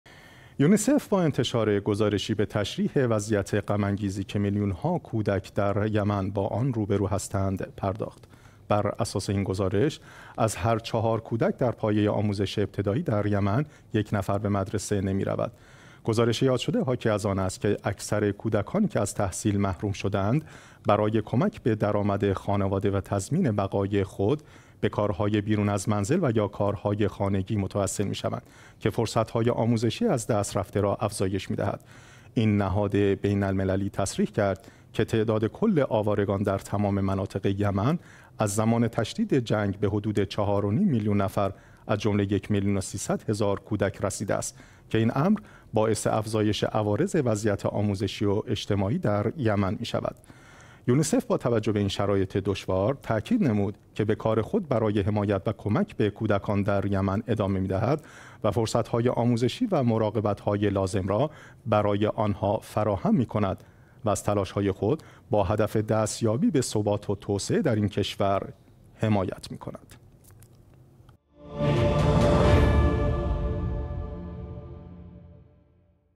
یونیسف: میلیون ها کودک در یمن به دلیل جنگ و پیامدهای آن مجبور به ترک تحصیل شدند
یونیسف با انتشار گزارشی به تشریح وضعیت غم انگیزی که میلیون‌ها کودک در یمن با آن روبرو هستند، پرداخت.

بر اساس این گزارش، از هر چهار کودک در پایه آموزش ابتدایی در یمن، یک نفر به مدرسه نمی رود.

گزارش یاد شده حاکی از آن است که اکثر کودکانی که از تحصیل محروم شده اند برای کمک به درآمد خانواده و تضمین بقای خود، به کارهای بیرون از منزل و یا کارهای خانگی متوسل می شوند که فرصت های آموزشی از دست رفته را افزایش می دهد.

این نهاد بین المللی تصریح کرد که تعداد کل آوارگان در تمام مناطق یمن از زمان تشدید جنگ به حدود چهار و نیم میلیون نفر از جمله یک میلیون و سیصد هزار کودک رسیده است که این امر باعث افزایش عوارض وضعیت آموزشی و اجتماعی در یمن می شود.

یونیسف با توجه به این شرایط دشوار، تاکید نمود که به کار خود برای حمایت و کمک به کودکان در یمن ادامه می دهد و فرصت های آموزشی و مراقبت های لازم را برای آنها فراهم می کند و از تلاش های خود با هدف دستیابی به ثبات و توسعه در این کشور حمایت می کند.